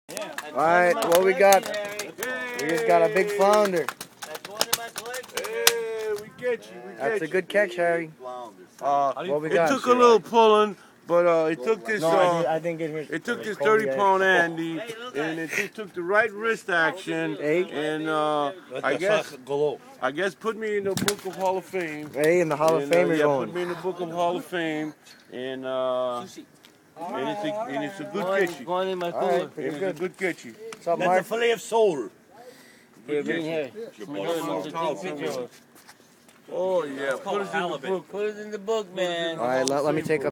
Gypsy fishing Harry gets a flounder
Harry caught a flounder. And that was not me who scrame at the start.